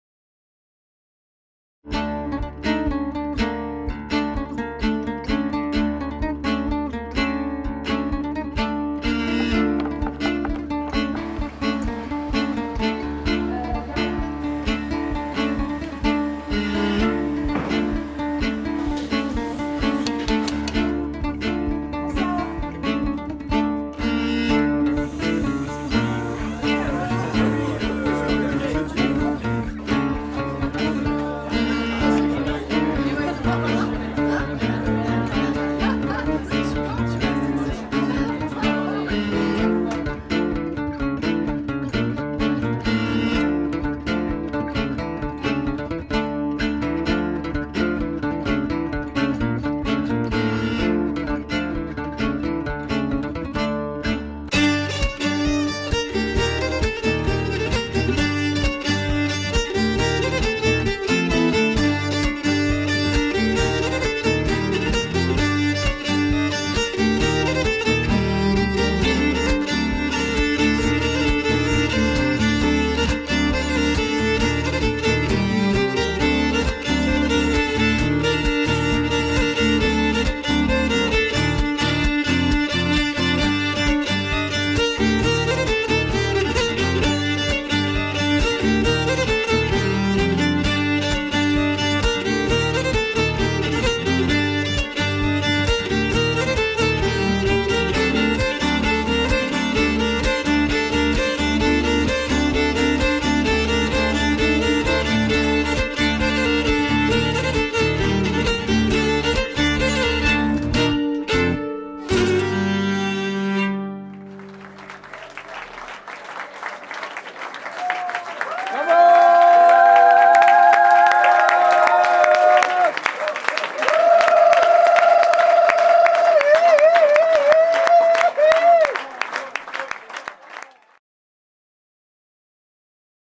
Retour sur le Bal du Printemps organisé par La Lanterne Prod.
After movie d'un week-end de bals organisé par La Lanterne Prod. avec Etienne Bordessoules, Trio Loubelya, Nøkkenoc et MoizBat'.